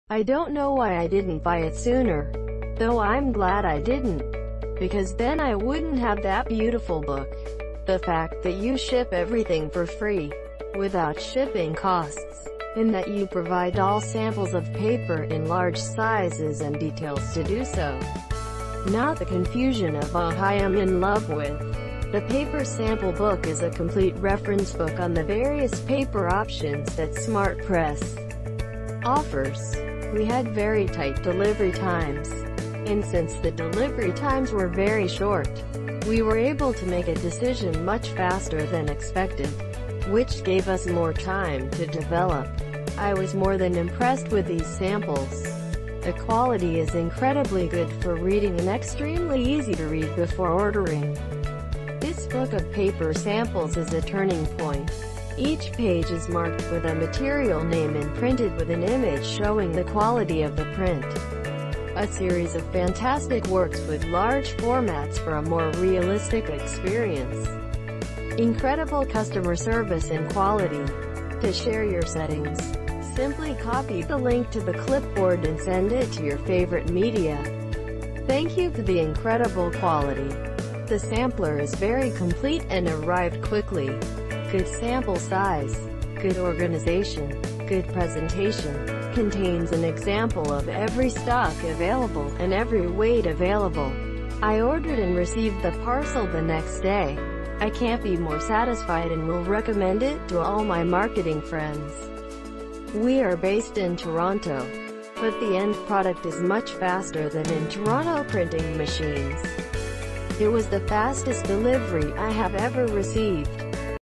Paper Sample Book - I don't know why I didn't buy it
I don't know why I didn't buy it sooner, though I'm glad I didn't, because then I wouldn't have that beautiful book! The fact that you ship everything for free, without shipping costs, and that you provide ALL samples of paper in large sizes and details to do so, not the confusion of UGH I am in love with. The Paper Sample Book is a complete reference book on the various paper options that Smartpress offers. Paper PaperSampleBook #|Smartpress